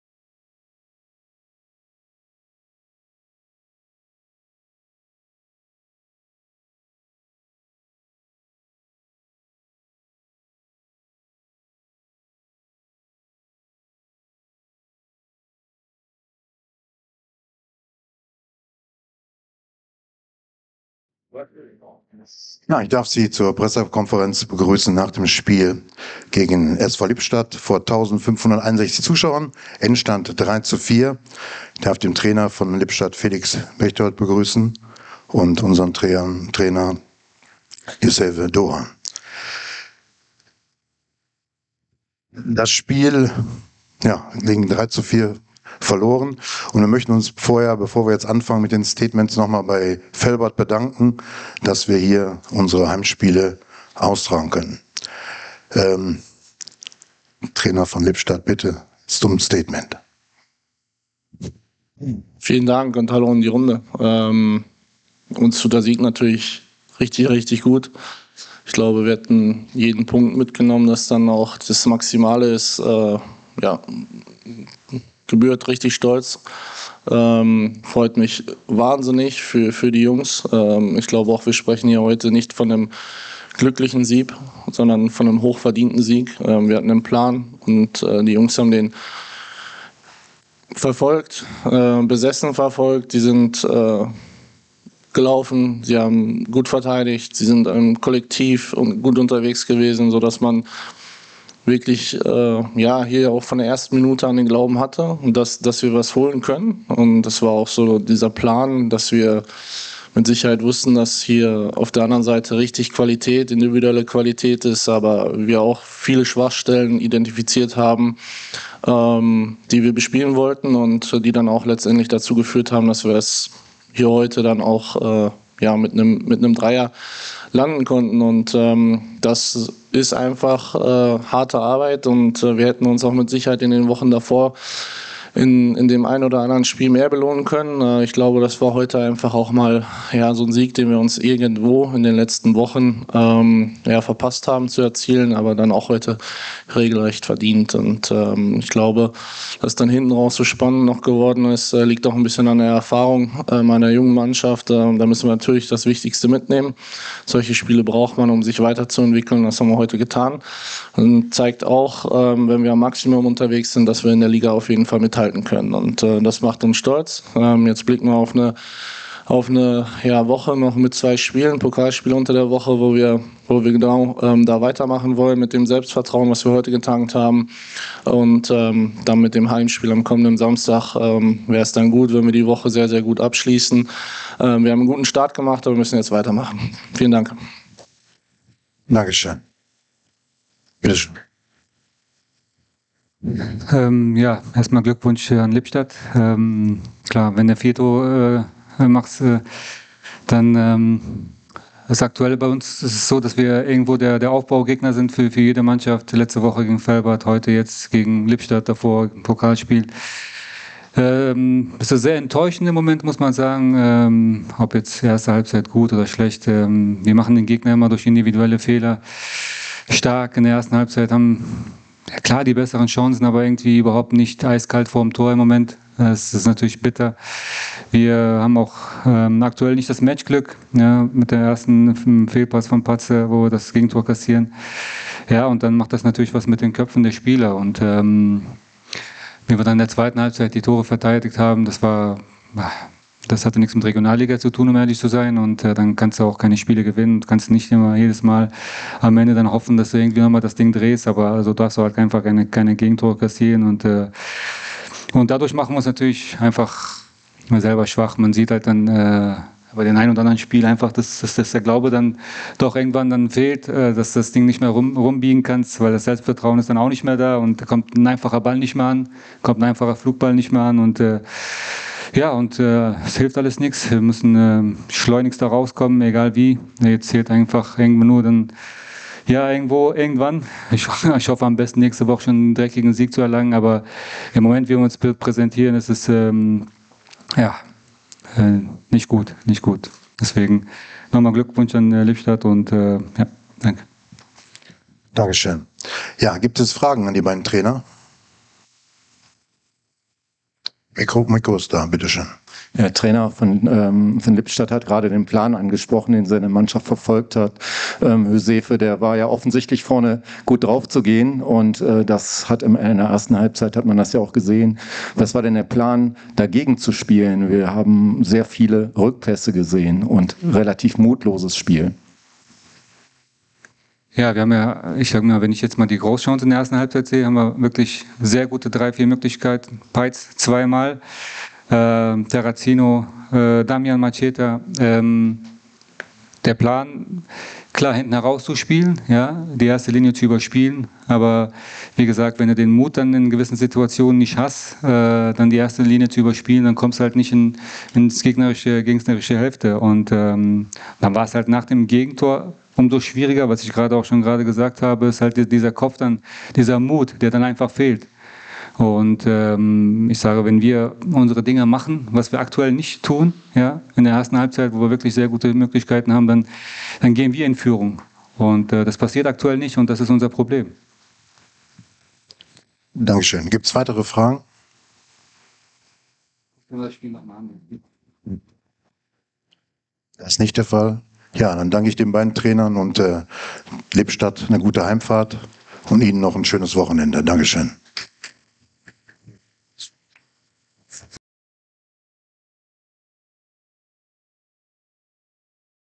REUPLOAD: Pressekonferenz nach Wuppertaler SV - SV Lippstadt 08 | Regionalliga 23/24
Wir begrüßen euch zur Pressekonferenz nach Wuppertaler SV - SV Lippstadt 08.

Ihr braucht Tickets?

Ihr habt Bock auf WSV-Merch?
oder auch im Fanshop direkt in der Rathaus Galerie.

Oder doch etwas aus der Streetwear Collection?